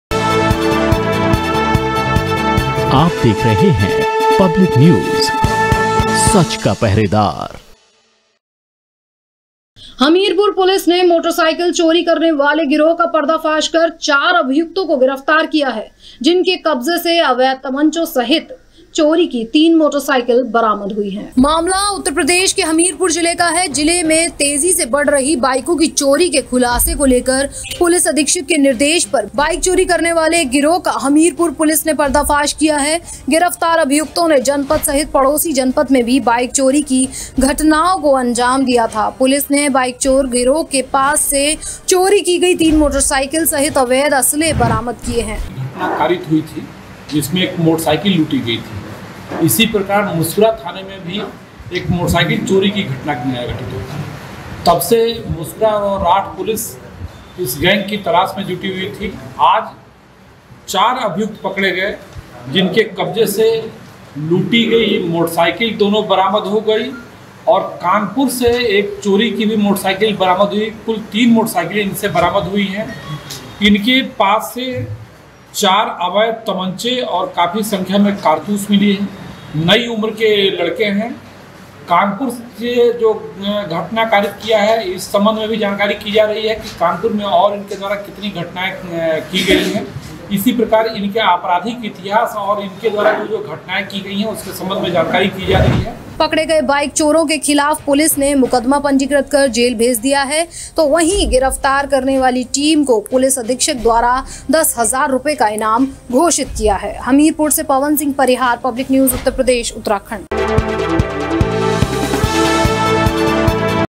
HAMIRPUR:बाइक चोर गिरोह का हमीरपुर पुलिस ने किया खुलासा
मामला उत्तर प्रदेश के हमीरपुर जिले का है, जिले में तेजी से बढ़ रही बाइकों की चोरी के खुलासे को लेकर पुलिस अधीक्षक के निर्देश पर, बाइक चोरी करने वाले एक गिरोह का हमीरपुर पुलिस ने पर्दाफाश किया है, गिरफ्तार अभियुक्तों ने जनपद सहित पड़ोसी जनपद में भी बाइक चोरी की घटनाओं को अंजाम दिया था, पुलिस ने बाइक चोर गिरोह के पास से चोरी की गई तीन मोटरसाइकिल सहित अवैध असलहे बरामद किए हैं, पकड़े गए बाइक चोरों के खिलाफ पुलिस ने मुकदमा पंजीकृत कर जेल भेज दिया है. तो वहीं गिरफ्तार करने वाली टीम को पुलिस अधीक्षक द्वारा 10 हजार रुपए का इनाम घोषित किया गया है.